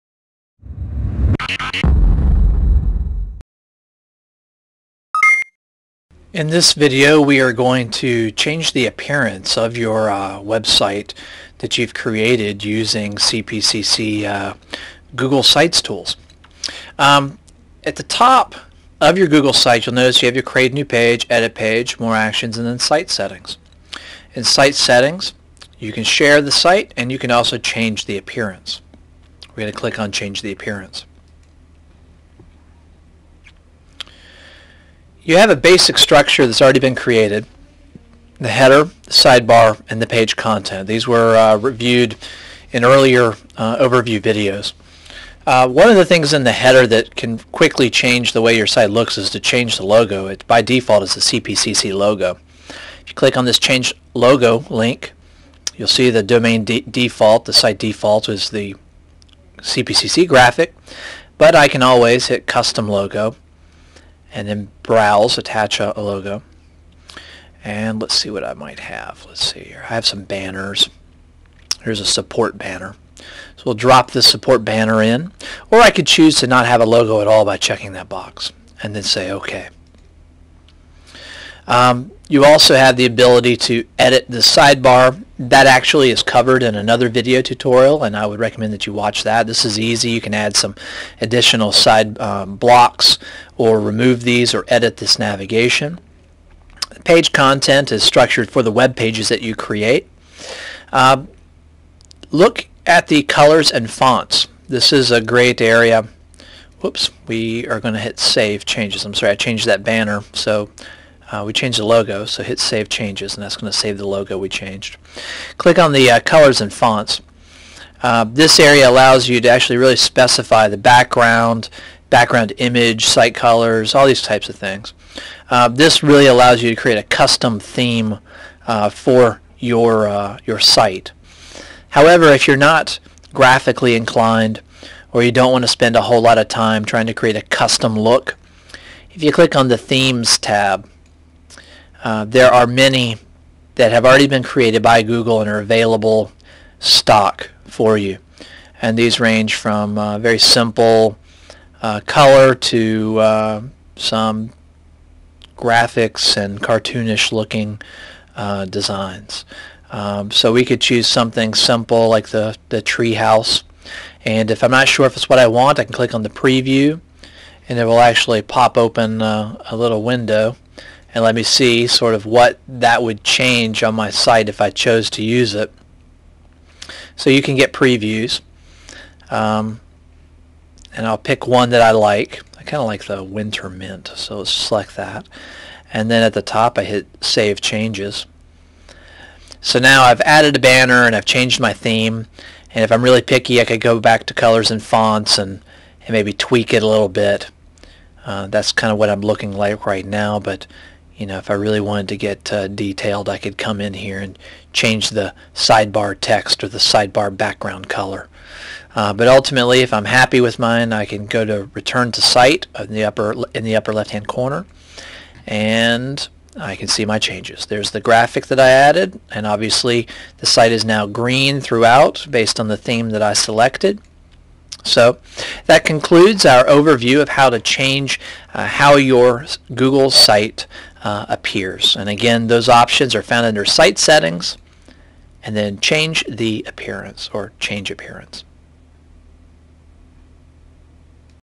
Change the appearance and design of your web site using CPCC Google Sites tools for students
This video will provide students with an overview on how to edit the design, layout, appearance and theme of web sites that they have created using CPCC Google Sites tools.